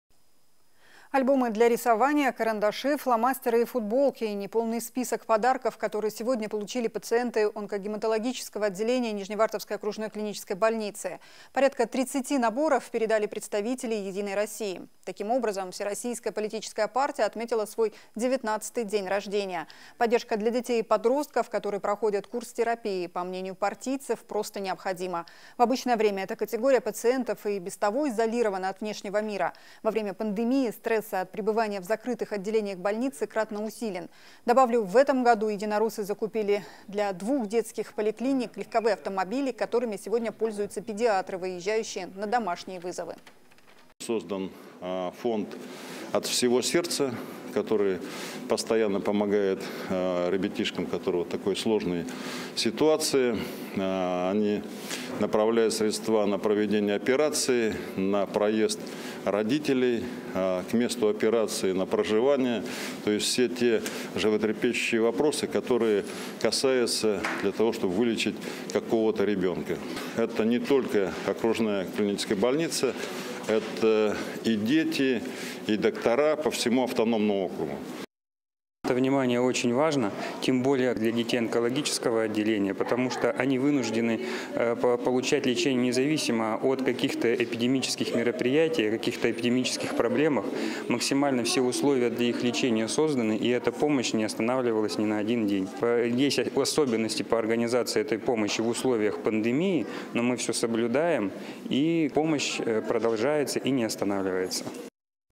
Единороссы передали подарки онкобольным детям в Нижневартовске
Альбомы для рисования, карандаши, фломастеры и футболки – неполный список подарков, которые сегодня получили пациенты онкогематологического отделения Нижневартовской окружной клинической больницы. Порядка тридцати наборов передали представители «Единой России». Таким образом всероссийская политическая партия отметила свой 19-й день рождения.

Смотрите выпуски "Вести-Югория" на телеканале Россия-1 ежедневно по будням:
вечером - 21.05
И в выходные: